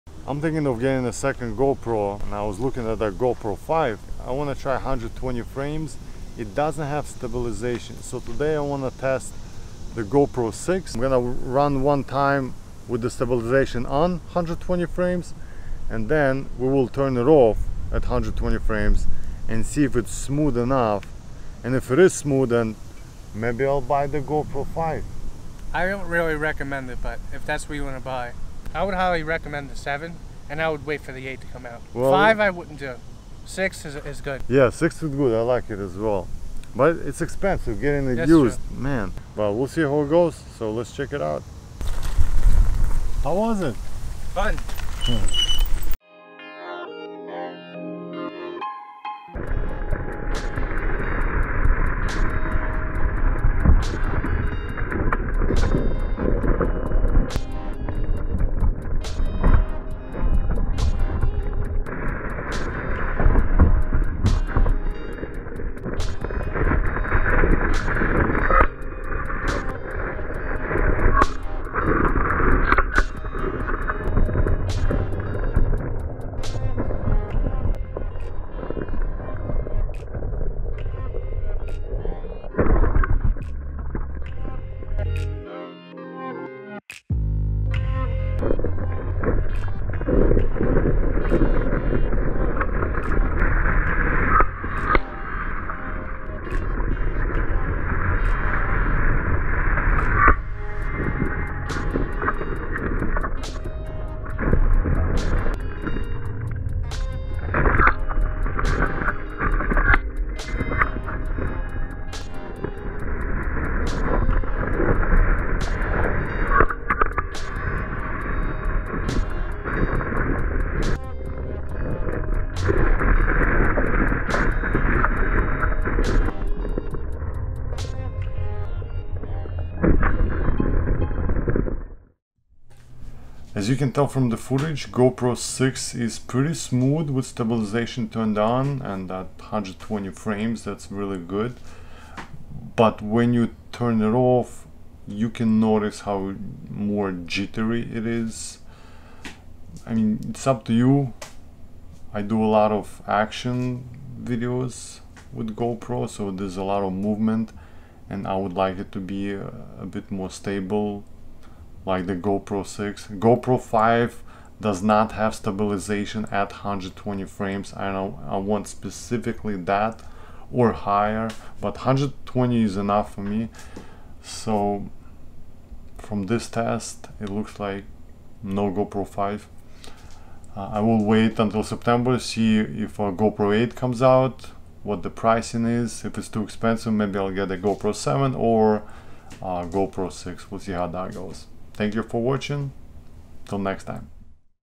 GOPRO 6 STABILIZATION TEST ON vs OFF eScooter Off Road
0:45 stabilization ON
1:25 stabilization OFF

Gopro5 is affordable & I thought it could be the perfect camera to mount for dangerous off roading with 120fps slow motion, but it lacks the stabilization at this frame rate &  as you can see the Gopro6 120fps with Stabilization OFF is very jittery.

thanks to Eric who participated & gave a valuable opinion, he owns GoPro5 6 & 7 & has a YouTube channel

- - - - G E A R & SETTINGS - - - - -
Sony a6300 w/ 10-18mm
Gopro6 @ 1080p 120fps 4x slow motion